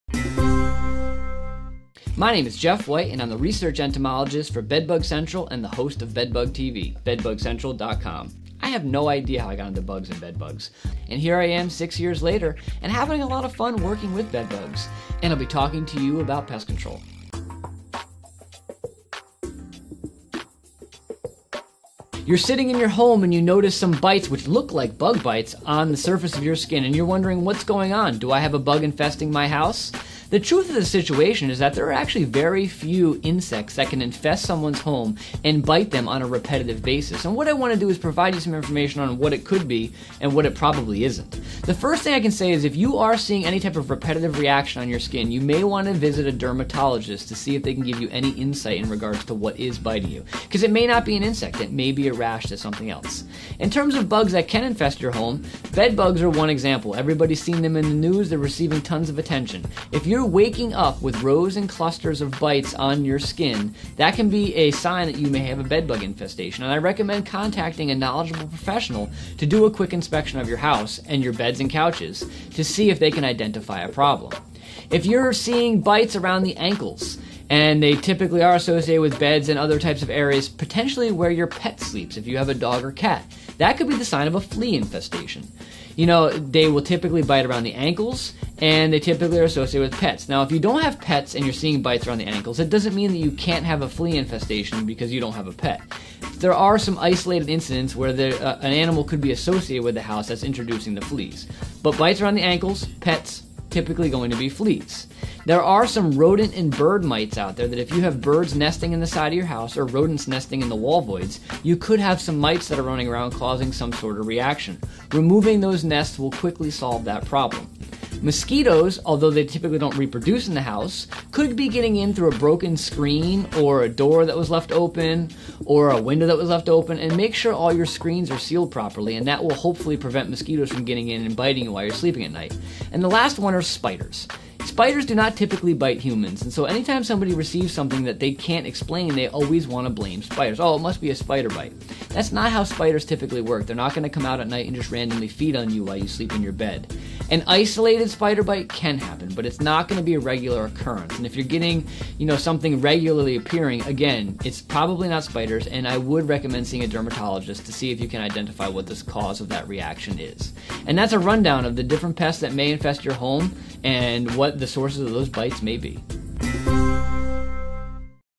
What Home Pests Bite? | Pest Control
My name is Jeff White and I'm the research entomologist for bedbug central and the host of bedbug TV bedbugcentral.com. I have no idea how I got into bugs and bedbugs and here I am 6 years later and  having a lot of fun working with bedbugs. And I'll be talking to you about pest control. You're sitting in your home and you notice some bites which look like bug bites, on the surface of your skin, and you're wondering what's going on, do I have a bug infesting my house? The truth of the situation is that there are actually very few insects that can infest someone's home and bite them on a repetitive basis and what I want to do is provide you with some information on what it could be, and what it probably isn't. The first thing I can say is that if you are seeing a repetitive reaction on your skin, you may want to visit a dermatologist to see if they can give you any insight in regards to what is biting you. Because it may not be an insect, it may be a rash or something else. In terms of bugs that can infest your home, bed bugs are one example, everybody's seen them in the news, they're receiving tons of attention. If you're waking up with rows and clusters of bites on your skin that can be a sign that you may have a bedbug infestation. And I recommend contacting a knowledgeable professional to do a quick inspection of your house and your beds and couches, to see if they can identify a problem. If you're seeing bites around the ankles and they typically are associated with beds and other types of areas, potentially where your pet sleeps, if you have a dog or cat, that could be the sign of a flea infestation. You know, they will typically bite around the ankles and they typically are associated with pets. Now if don't have pets and you're seeing bites around the ankles, it doesn't mean that you can't have a flea infestation, because you don't have a pet. There are some isolated incidents where an animal could be associated with the house that's introducing the fleas. But bites around they ankles, pets typically going to be fleas. There are some rodent and bird mites out there that if you have birds nesting in the side of your house, or rodents nesting in the wall voids, you could have some mites that are running around and causing some sort of reaction. Removing those nests will quickly solve that problem. Mosquitoes, although they typically don't reproduce in the house could be getting in through an broken screen or a door that was left open or a window that was left open. And make sure all your screens are sealed properly and that will hopefully prevent mosquitoes from getting in and biting you while you're sleeping at night. And the last one are spiders, spiders do not typically bit humans and so any time someone receives something they can't explain, they always want to blame spiders. Oh, it must be a spider bite. That's not how spiders typically work, they're not going to come out at night and just randomly feed on you while you sleep in your bed. An isolated spider bite can happen but it's not going to be a regular occurrence and if you're getting , you know something regularly appearing, again it's probably not spiders and I would recommend seeing a dermatologist to see if you can identify the cause of that reaction it is. And that's a rundown of the different pests that may infest your home and what the sources of those bites may be.